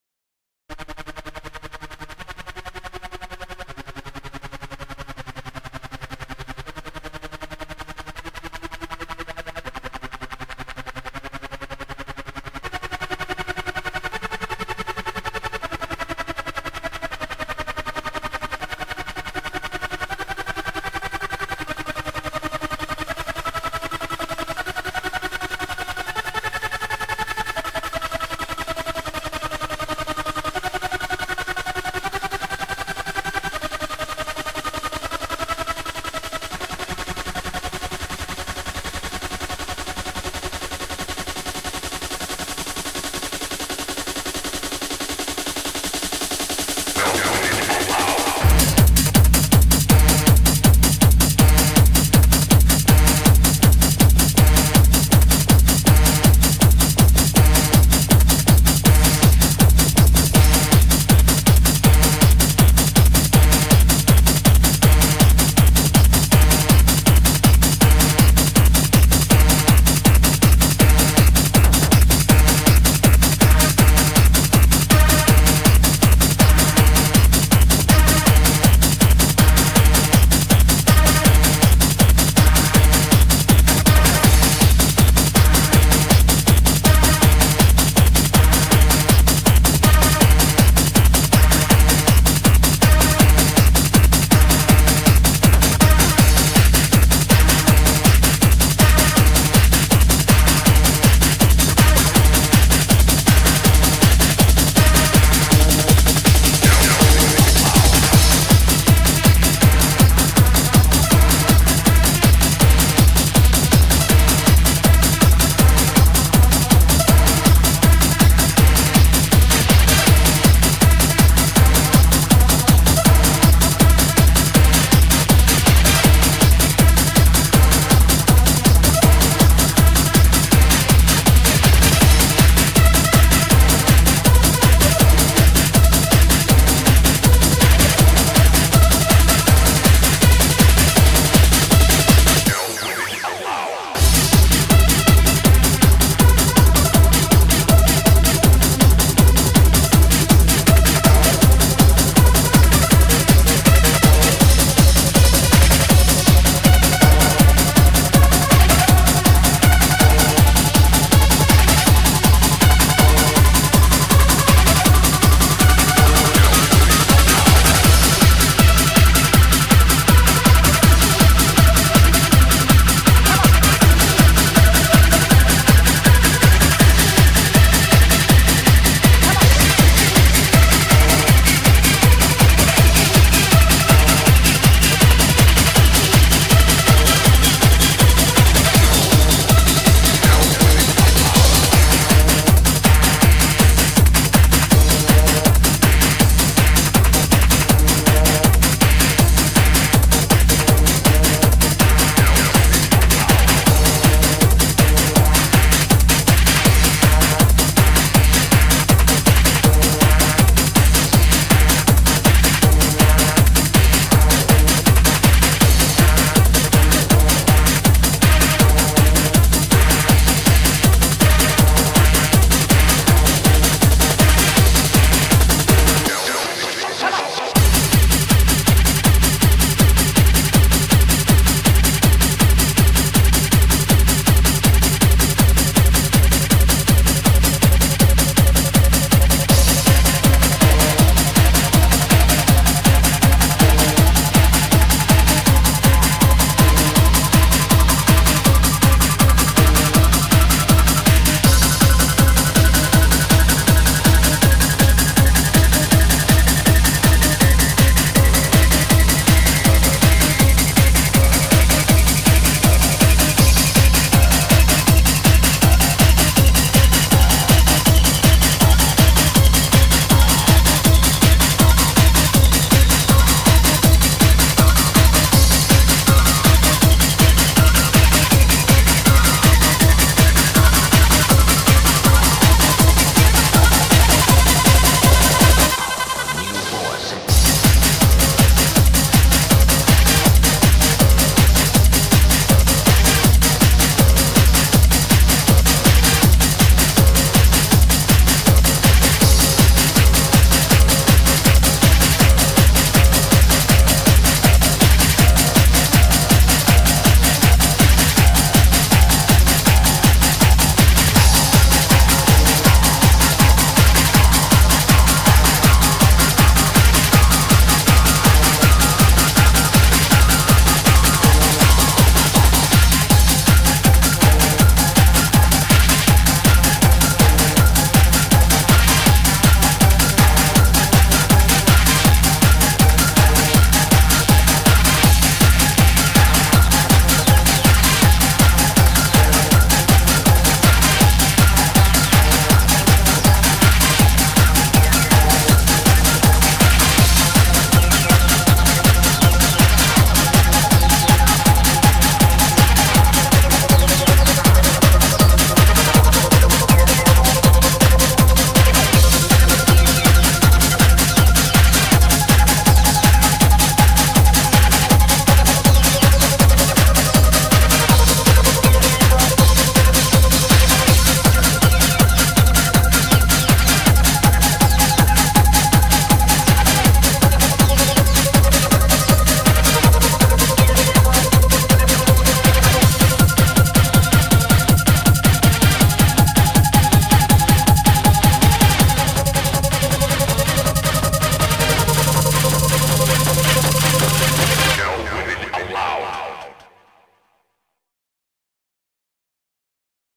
90's Hard Trance Mashup by TrancetasticWilza
a short mash up mix of some classic tunes :)